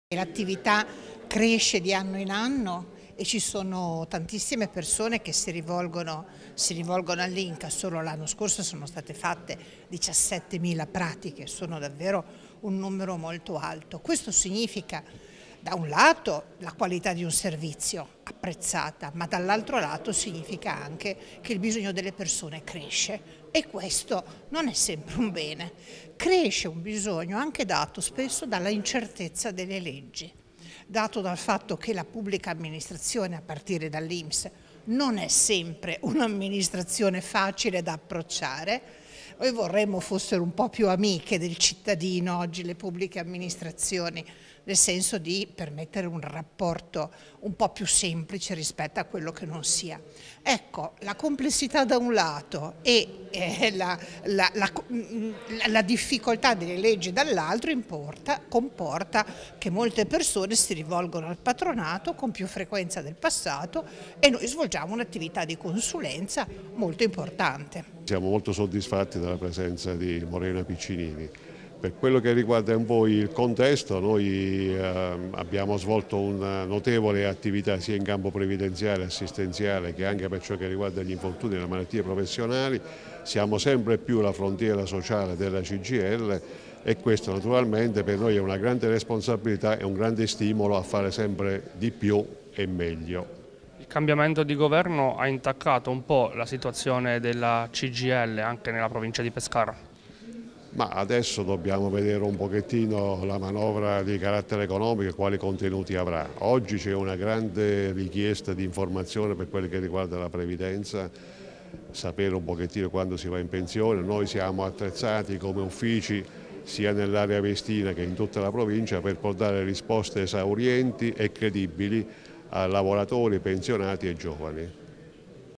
CGIL di Pescara presenta il bilancio sociale 2017
Presentato questa mattina presso la camera del lavoro CGIL di Pescara il bilancio sociale 2017 del patronato INCA CGIL provinciale di Pescara. Un avvicinamento al diciottesimo congresso nazionale di Bari previsto nel 2019. Presente al convegno anche il presidente nazionale Morena Piccinini.